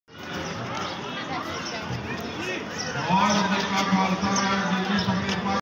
Sikh Yatris visit Gurudwara Janam Asthan Nankana Sahib. (2)
Sikh Yatris from different countries including India and Canada visit Gurudwara Janam Asthan Nankana Sahib, Pakistan to celebrate Besakhi Festival on April 15, 2022.